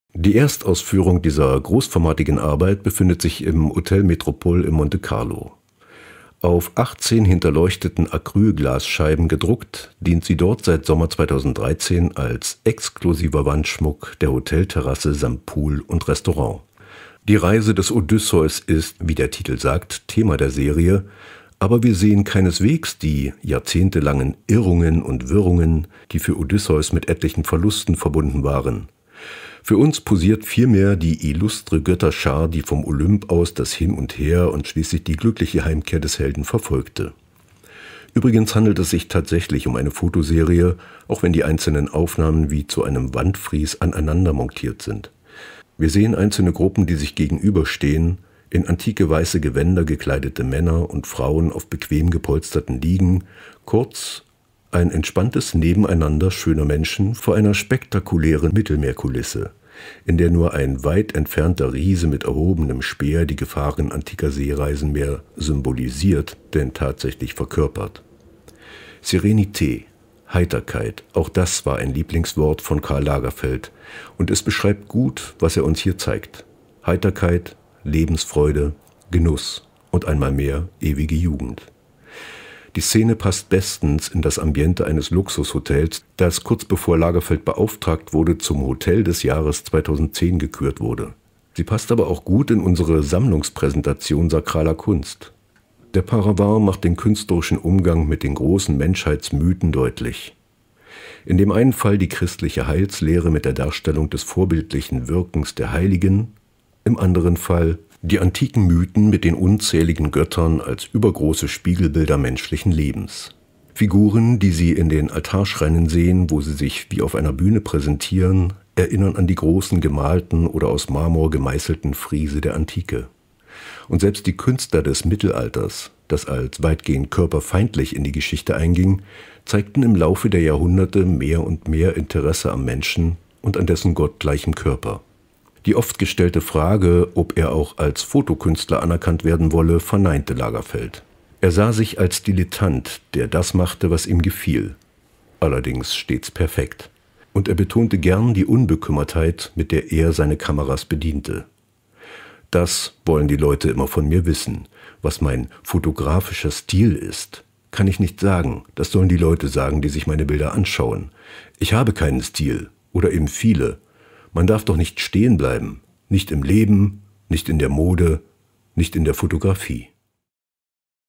closedbutopen XVI wird openbutsafe I
Seit gestern und ganz neu zu sehen ist Karl Lagerfelds Interpretation der Geschichten um Odysseus. Nach mehr als zwei Jahren Aufzugsbauarbeiten sind unsere Gotischen Gewölbe wieder zugänglich - mit einer beeindruckenden Intervention der aktuellen Sonderausstellung. Lasst euch überraschen.